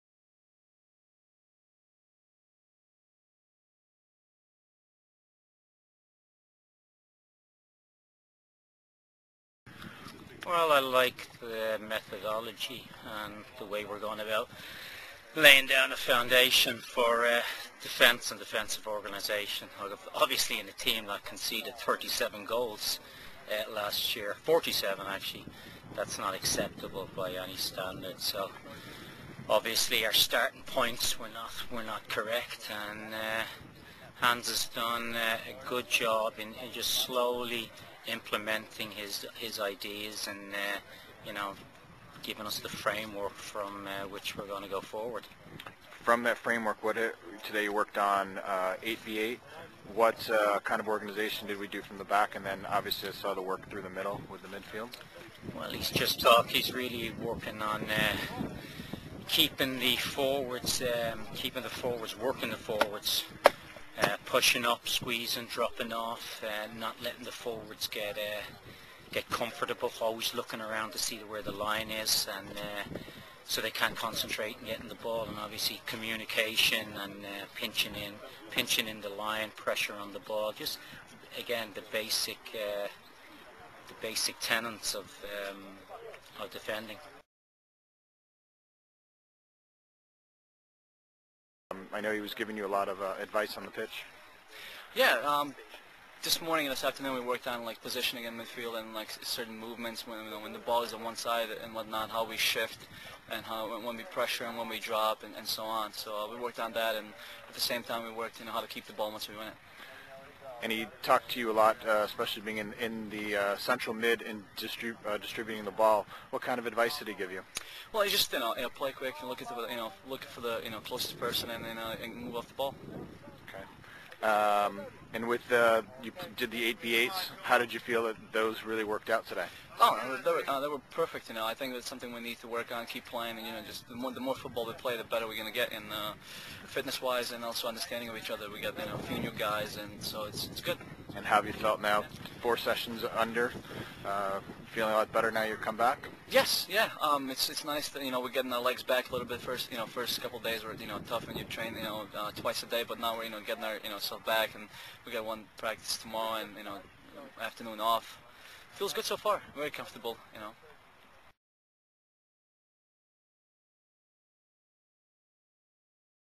New York Red Bulls: Day 3 La Manga Spain
It's Day Three in La Manga, Spain, for the New York Red Bulls. Goalkeeping coach Des McAleenan and midfielder Sinisi Ubiparipovic give their thoughts after the two training sessions.